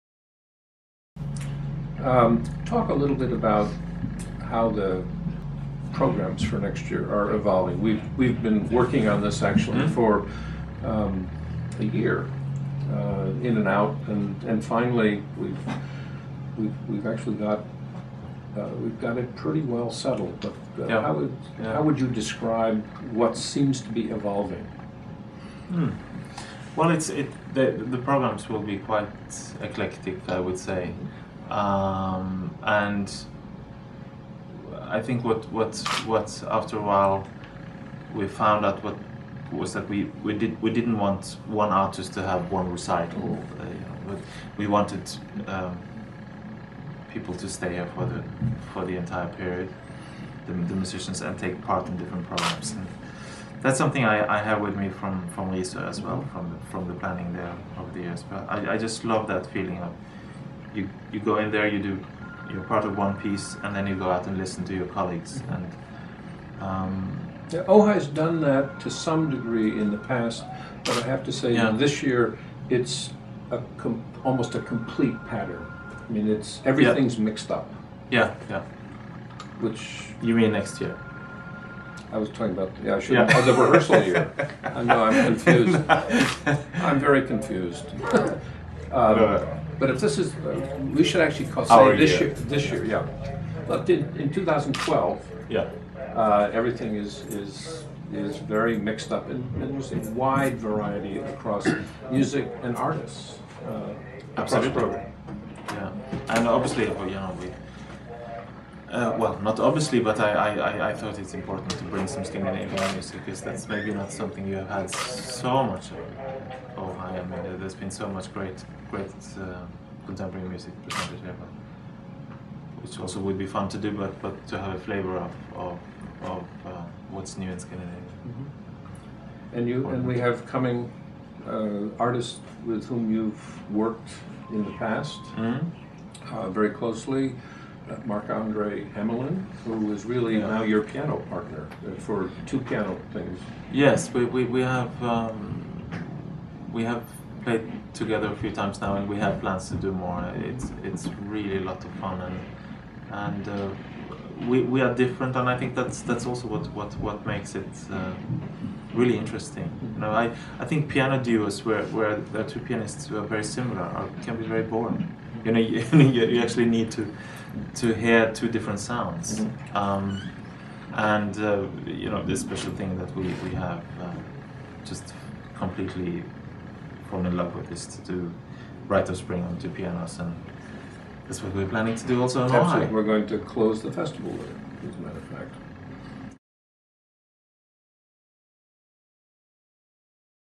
Ojai 2012 Trailer: Leif Ove PT 2
2012 Ojai Music Festival Music Director Leif Ove Andsnes talks about programming for the 2012 Festival in Ojai, California, with artistic director Thomas W. Morris